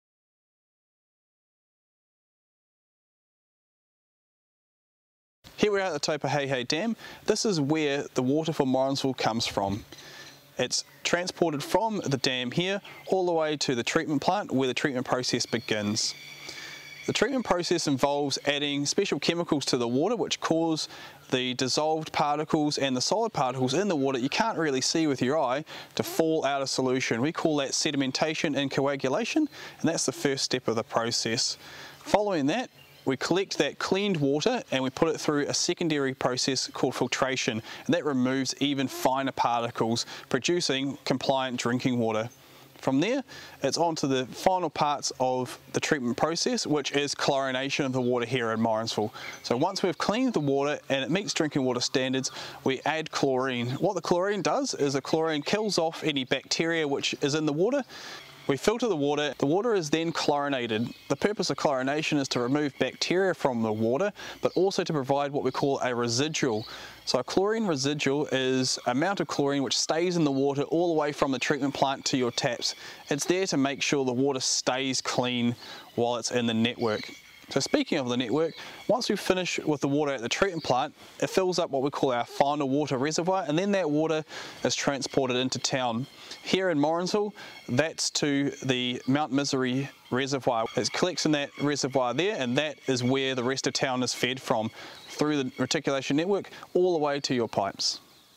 LTP water challenges - What happens to water before it gets delivered to your tap
Water and waste water manager Karl Pavlovich talks about what's involved to provide safe, healthy drinking water for residents on town supply.